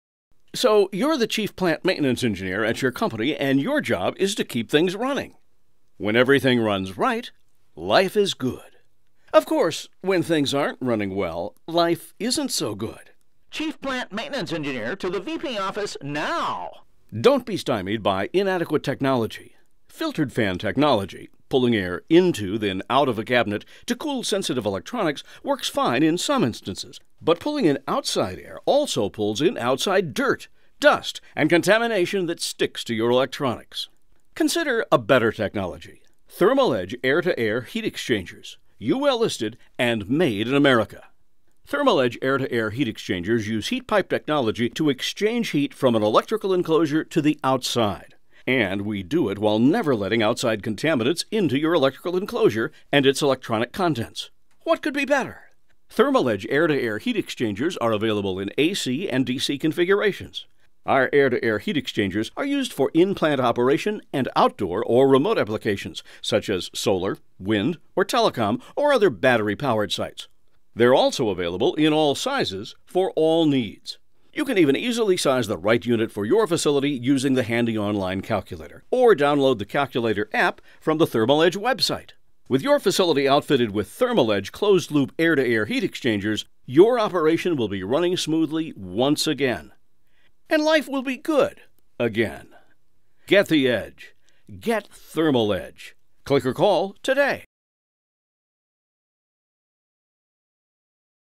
Industrial Air to Air Heat Exchanger-Best Efficiency by Thermal Edge Inc.
Industrial Air to Air Heat Exchanger from Thermal Edge Inc.

Thermal Edge Air to Air Heat Exchanger is a closed loop cooling system which employs heat pipe technology to exchange heat from an electrical enclosure to the outside. Where ambient temperatures are suitable for heat pipes, our air to air heat exchanger design are the most efficient method of cooling as the waste heat is the engine which drives the system. The only power requirement is to operate two circulating fans or blowers. All units are available in NEMA Type 12, 4 and 4X.

Components are Failing inside electrical enclosures when using Filtered Fan Packages CONSIDER A BETTER SOLUTION - AIR TO AIR HEAT EXCHANGER industrial calculations

Save Money NOW, Buy Instantly Online!

Stop Your Components from Failing inside electrical enclosures!

* Filter-free Operation

* Full use of insulation to preserve the cold fraction

Available in:
           -120V, 230V, 24VDC and 48VDC

           - NEMA Type 12, 4 and 4X

           - Hazardous Location Units available